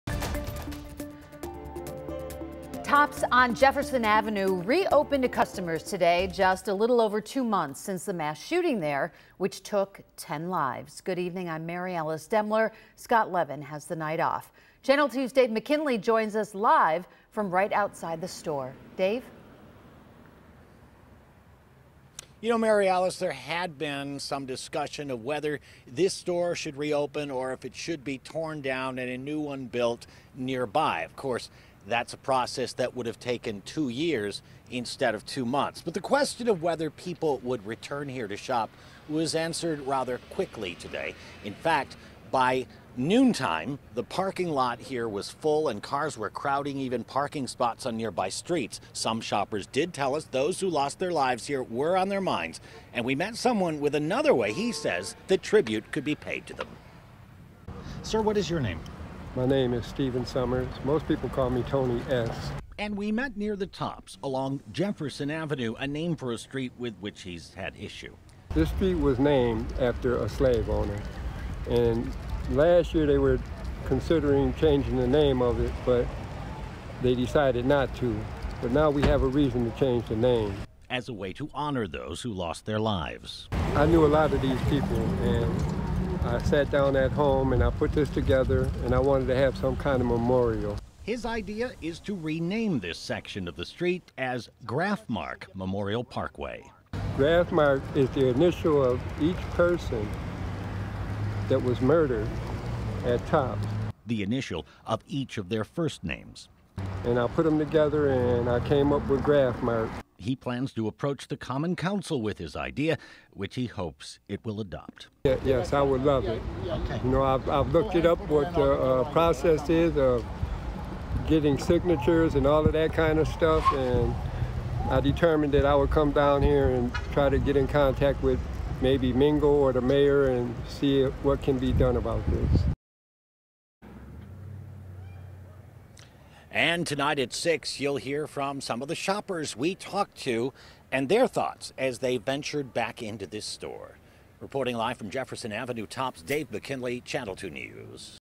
Tops On Jefferson Reopens
noontime the parking lot behind me was full, and cars were crowding the parking spots on nearby side streets.